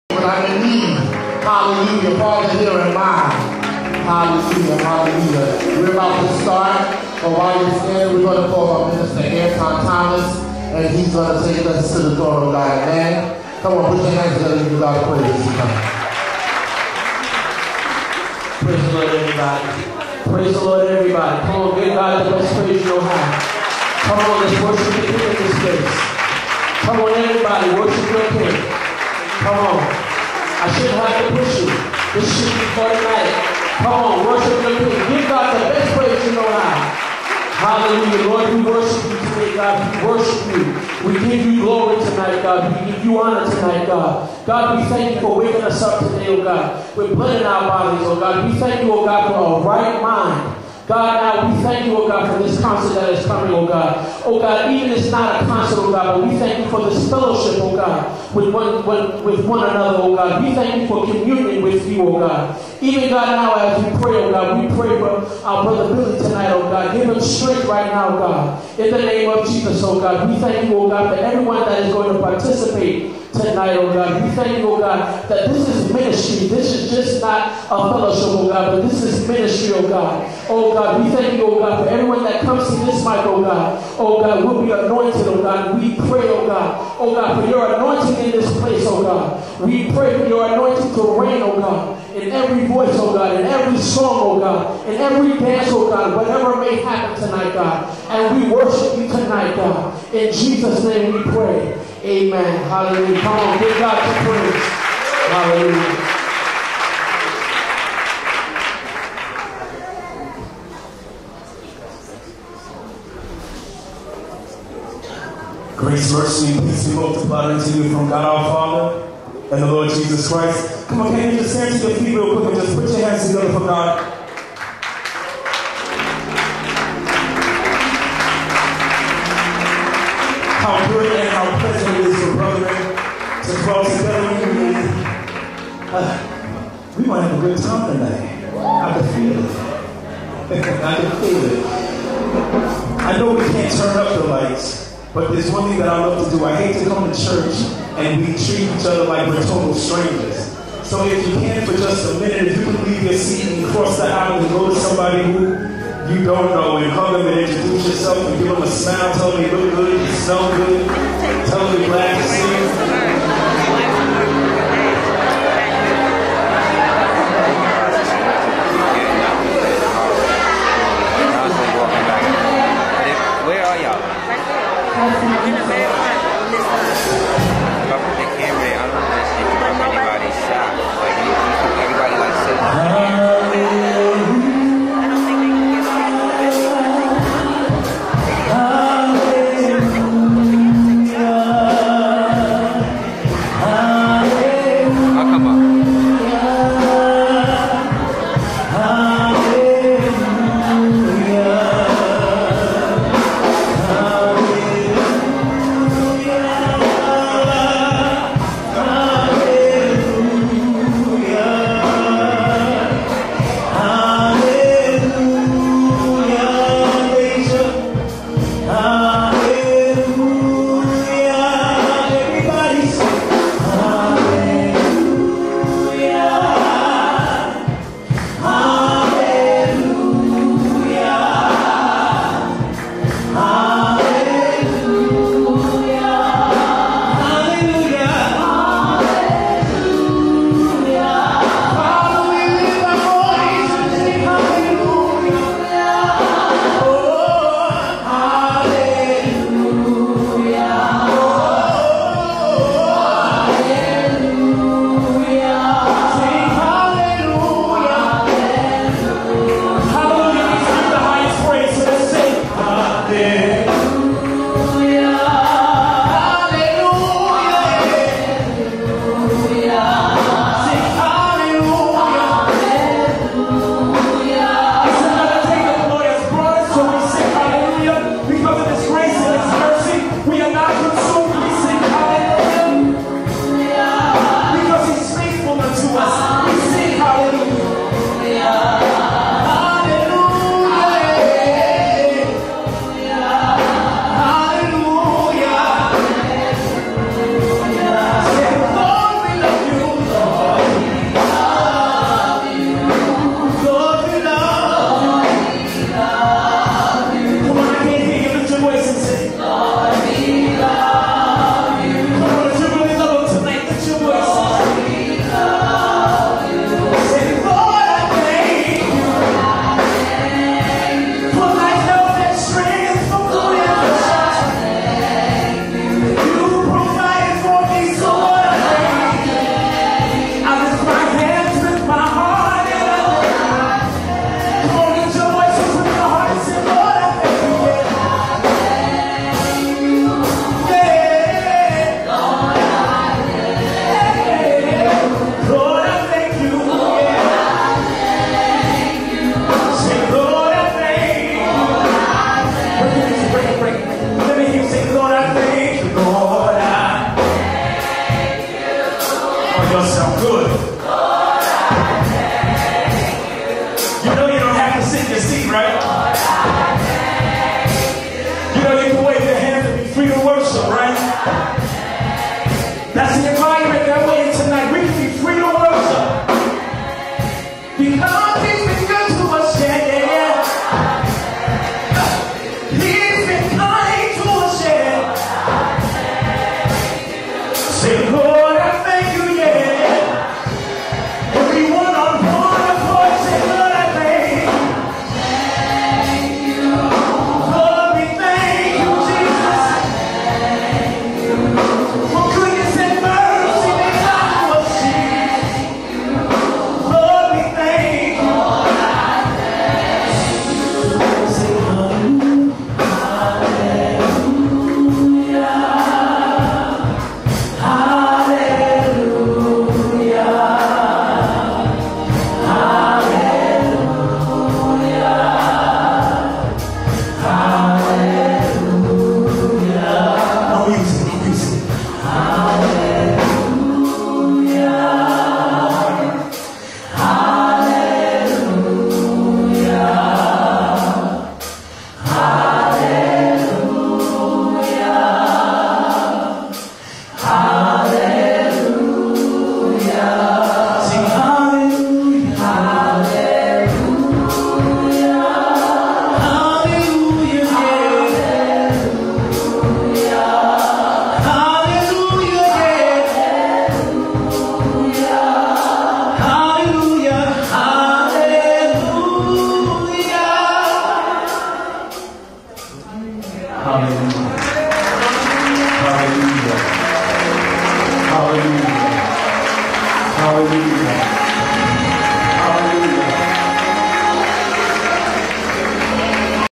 BEGINNING OF SHOW [WORD FROM BILLY, ANTON THOMAS, AND performance from ABRAHAM BLUE]
SiMunye DANCE MINISTRY and Mr. William Berry {Billy} Presented a great late Winter Showcase with an array of AMAZING and Extra TALENTED ARTISTS and Individuals.  Hosted by BETTINA PENNON, who did an Awesome job.
Producer: William Berry
Camera/Editor: Rashad Worthy & "Ya Man" BAM courtesy of THE WORTHY FOUNDATION